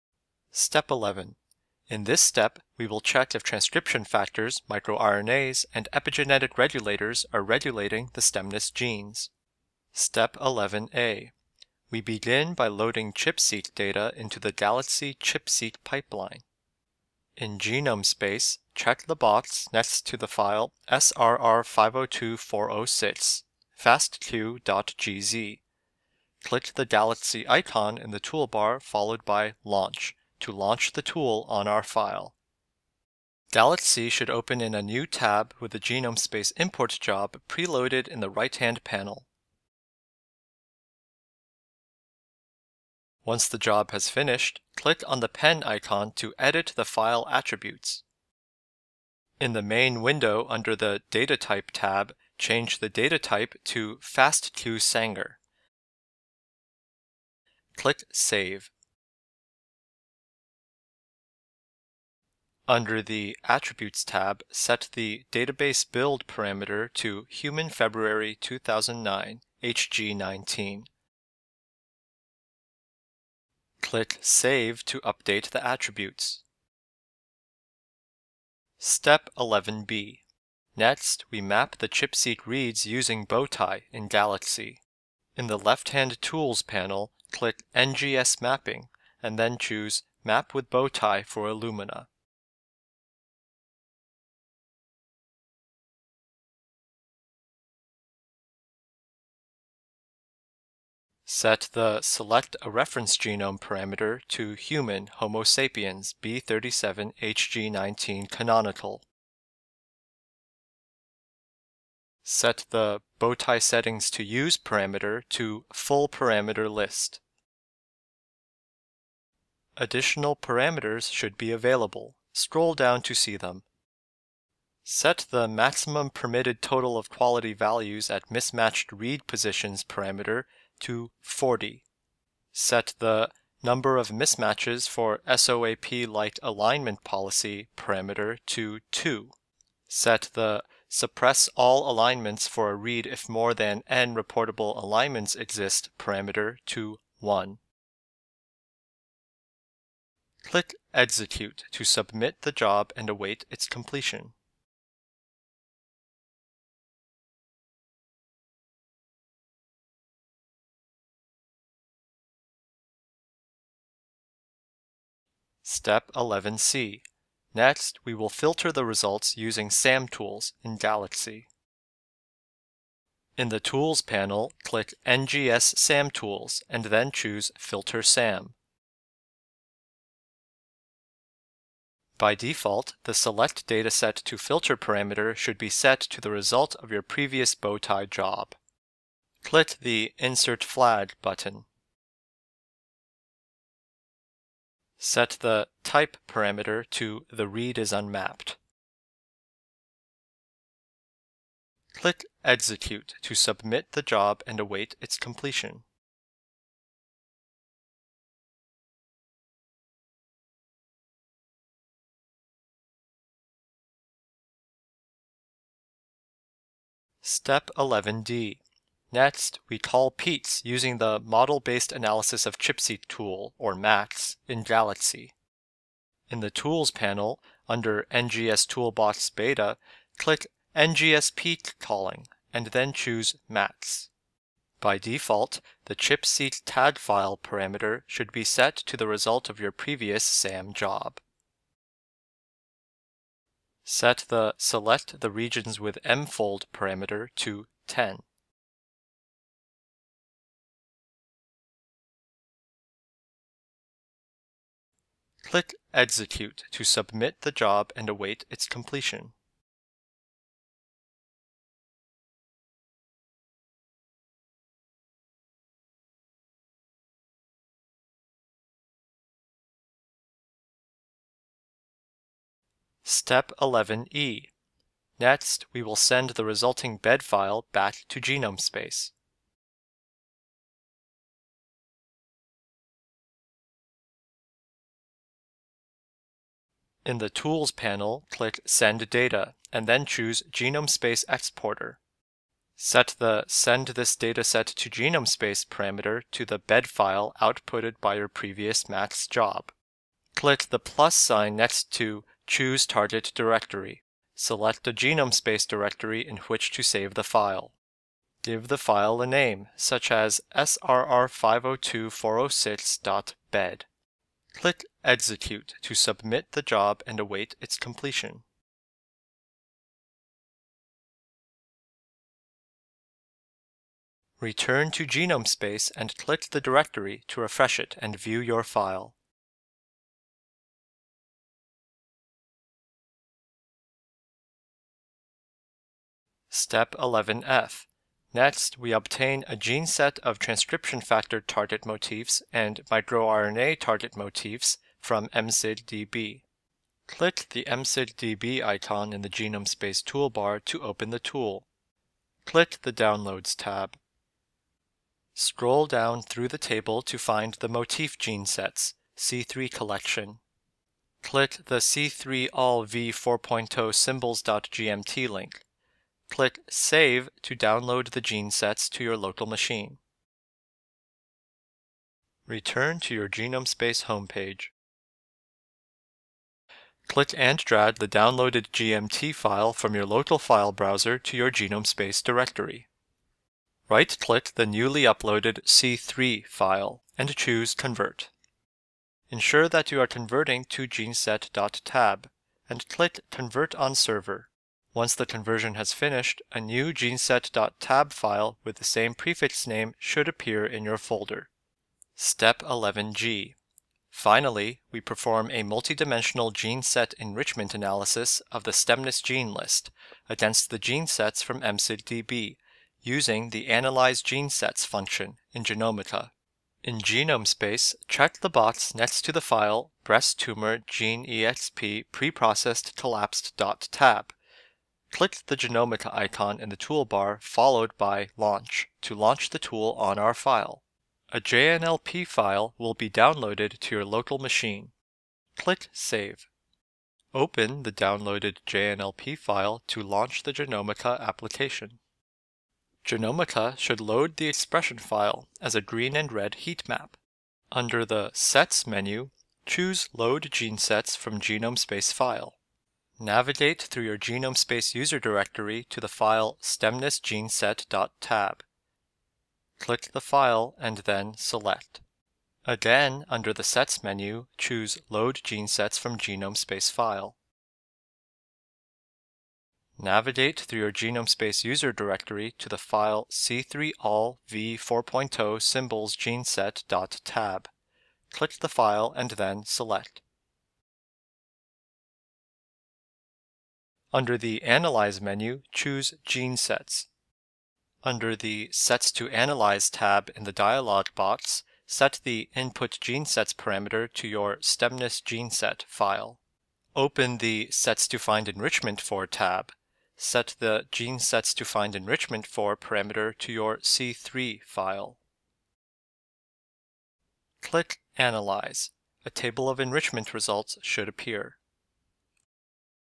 Driving Biological Project - Step 11
This is Step 11 of the driving biological project, "Identifying Gene Regulatory Networks in Human Cancer Stem Cells"

Step 11. Check if transcription factors, microRNAs and epigenetic regulators are regulating the stemness genes

Step 11A. Loading ChIP-Seq data into the Galaxy ChIP-Seq pipeline.
In GenomeSpace, check the box next to the file, "SRR502406.fastq.gz". Click the Galaxy icon, followed by "Launch". Galaxy will with the GenomeSpace import job preloaded in the right hand panel. Once the job finishes, click the pen icon to edit the file attributes. In the main window, under "Datatype", change the data type to "fastqsanger". Click "Save". Under the attributes tab, set the "Database/Build" parameter to "Human Feb. 2009 (HG19)". Click "Save" to update attributes.

Step 11B. Map the ChIP-Seq reads using Bowtie in Galaxy.
In the Tools panel, click "NGS Mapping" and then "Map with Bowtie for Illumina". Set the "Select a reference genome" parameter to "Human (Homo Sapiens) (b37) hg19 Canonical". Set the "Bowtie settings to use" parameter to "Full parameter list". Additional parameters should be available. Scroll down to see them. Set the "Maximum permitted total of quality values at mismatched read positions" parameter to "40". Set the "Number of mismatches for SOAP-like alignment policy" parameter to "2". Set the "Suppress all alignments for a read if more than N reportable alignments exist" parameter to "1". Click "Execute" to submit the job and await its completion.

Step 11C. Filter results using SAM Tools in Galaxy.
In the Tools panel click "NGS SAM Tools", and then choose "Filter SAM". By default, the "Select dataset to filter" parameter should be set to the result from your previous Bowtie job. Click the "Insert flag" button. Set the "Type" parameter to the "The read is unmapped". Click "Execute" to submit the job and await its completion.

Step 11D. Call peaks using the Model-based Analysis of ChIP-Seq tool, or MACS, in Galaxy.
In the Tools panel under "NGS Toolbox Beta", click "NGS Peak Calling", and then choose "MACS". By default, the "ChIP-Seq Tag File" parameter should be set to the result of your previous SAM job. Set "Select the regions with MFOLD" parameter to 10. Click "Execute" to submit the job and await its completion.

Step 11E. Send the resulting BED file back to GenomeSpace.
In the Tools panel, click "Send Data", and then choose "GenomeSpace Exporter". Set the "Send this dataset to GenomeSpace" parameter to the BED file outputted by your previous MACS job. Click the plus sign next to "Choose Target Directory". Select a GenomeSpace directory in which to save the file. Give the file a name, such as "SRR502406.bed". Click "Execute" to submit the job and await its completion. Return to GenomeSpace and click the directory to refresh it and a view your file.

Step 11F. Obtain a gene set of transcription factor target motifs and microRNA target motifs from MSigDB.
Click the MSigDB icon in the GenomeSpace toolbar to open the tool. Click the "Downloads" tab. Scroll down through the table to find the motif gene sets (C3) collection. Click the "c3.all.v4.0.symbols.gmt" link. Click "Save" to download the gene sets to your local machine. Return to your GenomeSpace home page. Click and drag the downloaded GMT file from your local file browser to your GenomeSpace directory. Right-click the newly uploaded C3 file and choose "Convert". Ensure that you are converting to "geneset.tab", and click "Convert on Server". Once the conversion has finished, a new geneset.tab file with the same prefix name should appear in your folder.

Step 11G. Perform a multi-dimensional gene set enrichment analysis of the stemness gene list against the gene sets from MSigDB using the Analyze Gene Sets function in Genomica.
In GenomeSpace check the box next to the file, "breasttumor.geneexp.preprocessed.collapsed.tab". Click the Genomica icon in the toolbar, followed by "Launch", to launch the tool on our file. A JNLP file will be downloaded your local machine. Click "Save". Open the downloaded JNLP file to launch the Genomica application. Genomica should load the expression file as a green and red heatmap. Under the "Sets" menu, choose "Load Gene Sets from GenomeSpace File...". Navigate through your GenomeSpace user directory to the file, "stemness.geneset.tab". Click the file and then "Select". Again, under the "Sets" menu, choose "Load Gene Sets from GenomeSpace File...". Navigate through your GenomeSpace user directory to the file, "c3.all.v4.0.symbols.geneset.tab". Click the file and then "Select". Under the "Analyze" menu, choose "Gene Sets". Under the "Sets to Analyze" tab in the dialog box, set the "input gene sets" parameter to your stemness.geneset file. Open the "Sets to Find Enrichment For" tab. Set the "Gene sets to find enrichment for" parameter to your c3 file. Click "Analyze". A table of enrichment results should appear.